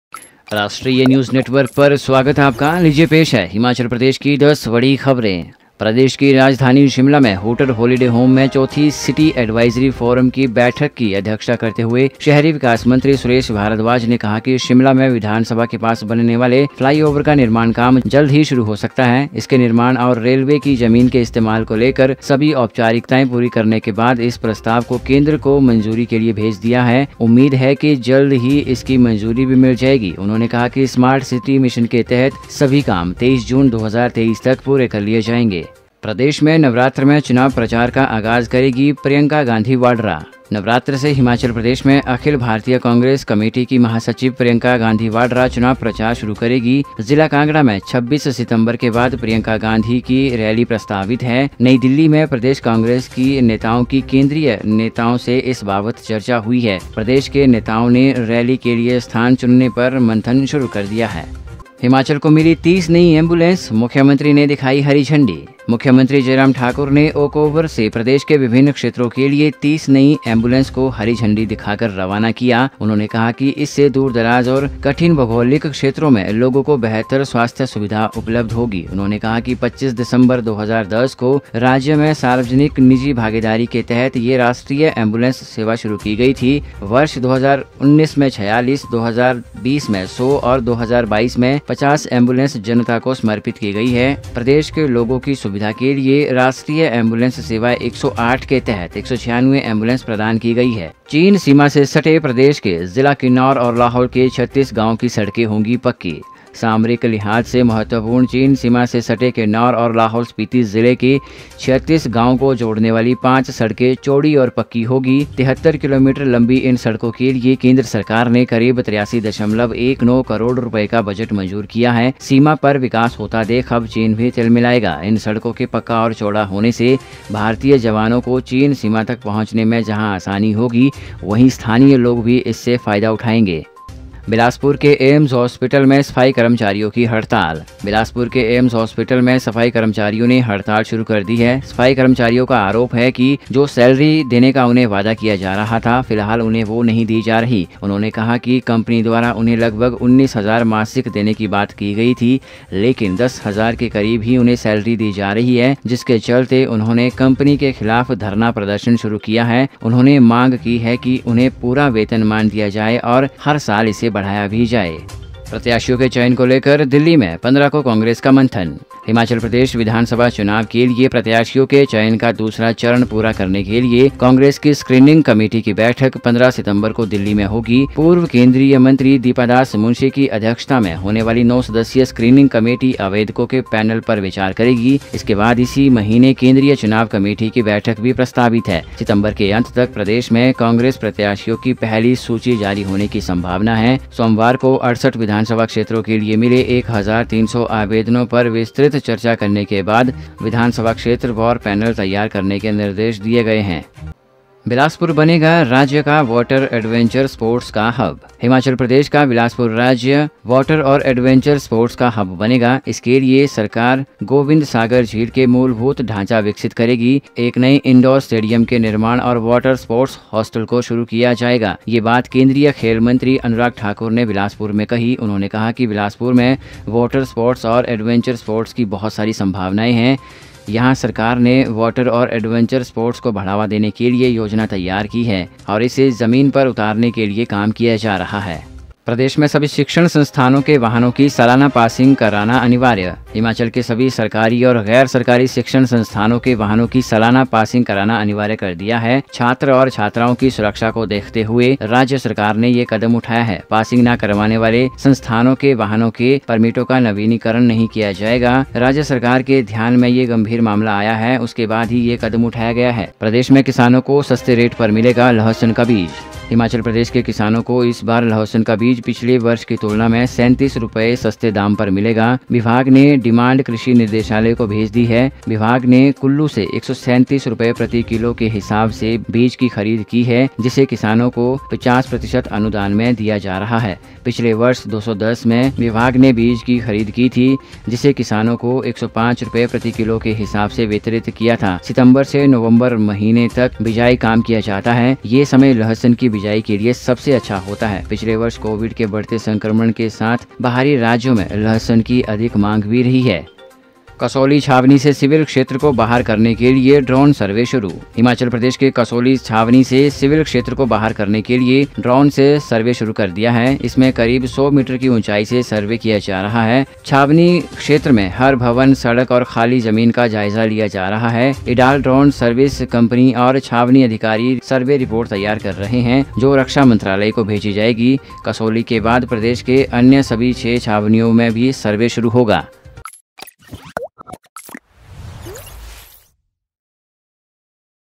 Today Breaking News Himachal | हिमाचल की 10 बड़ी खबरें | 8 Sep 2022 | Himachal Ki Badi Khabren
प्रदेश मे किसानों को सस्ते रेट पर मिलेगा लहसुन का बीज
बिलासपुर बनेगा राज्य का वाटर एडवेंचर स्पोर्ट्स का हब
बिलासपुर के एम्स हॉस्पिटल में सफाई कर्मचारियों की हड़ताल
कसौली छावनी से सिविल क्षेत्र को बाहर करने के लिए ड्रोन सर्वे शुरू